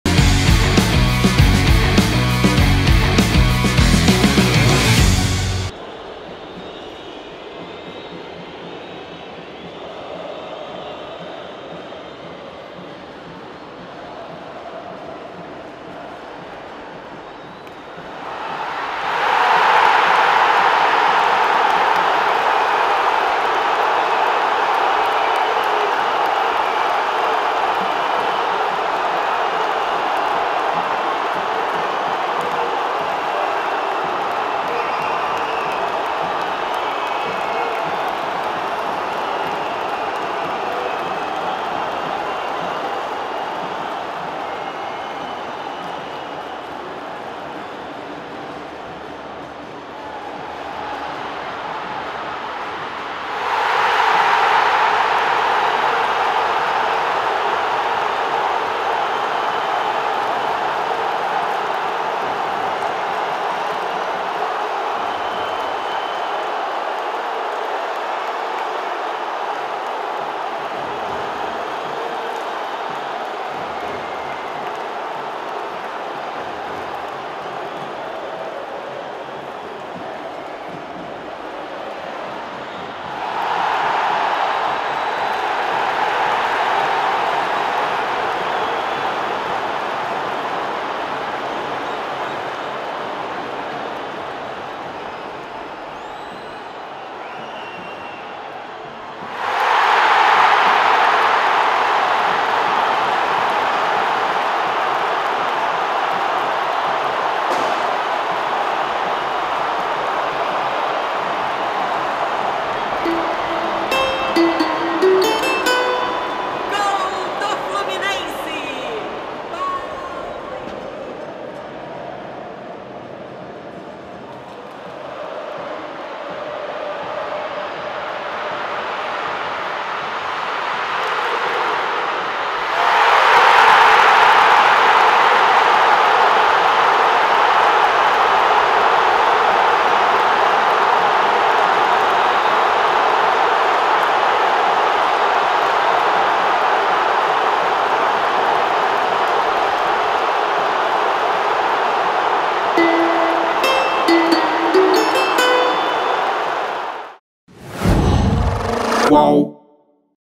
Fluminense reage e empata com Botafogo em jogo eletrizante no Maracanã
Conteúdo publicado:
Cliente: UOL
Serviço: Captação de imagens e edição
Produção, imagens e edição: Léo Nunes